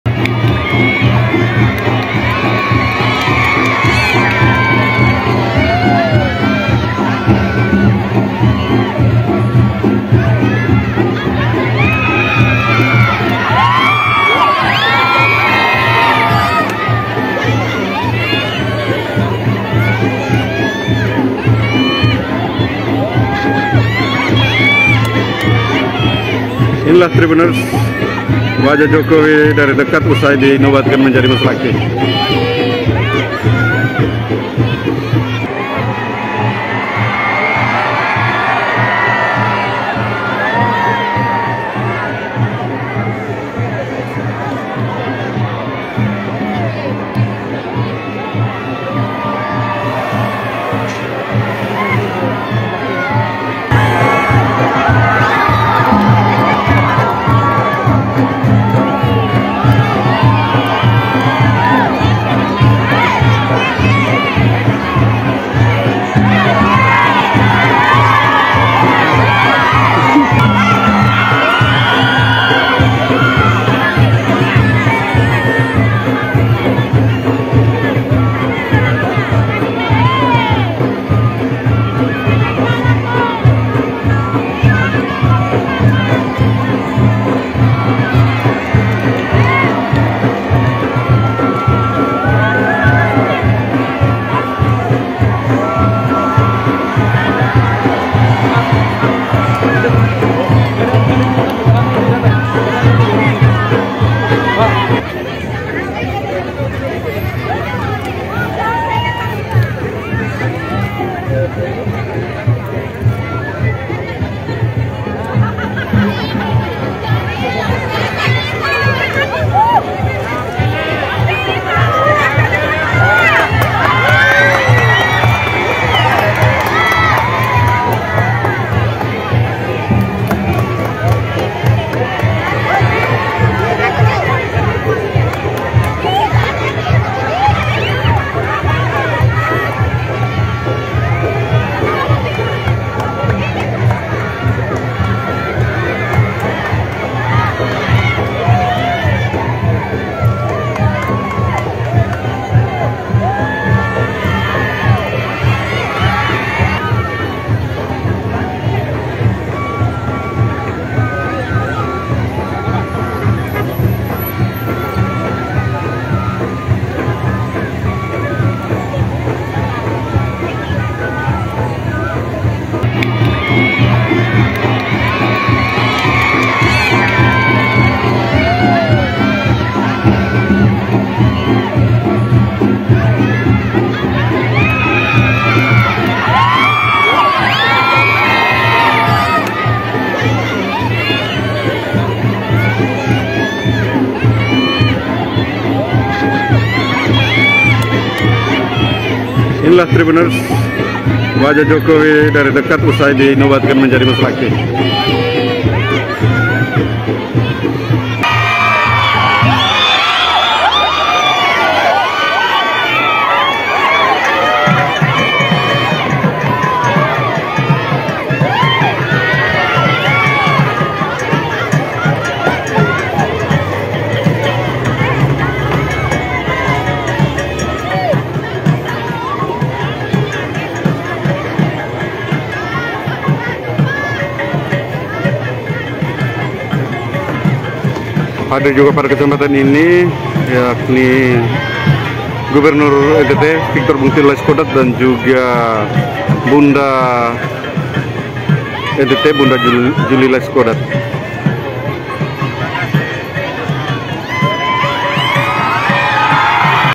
Wajah Presiden Jokowi usai Dinobatkan Menjadi Mosalaki Ende NTT
POS-KUPANG.COM - Presiden Jokowi dinobatkan menjadi Mosalaki Ende usai upacara Peringatan Hari Pancasila di Kota Ende, NTT, Rabu, (01/06/2022).

Penobatan itu dilakukan saat Presiden Jokowi kunjungi Taman Renungan Bung Karno.

Jurnalis: Tommy Nulangi
Editor: Dhev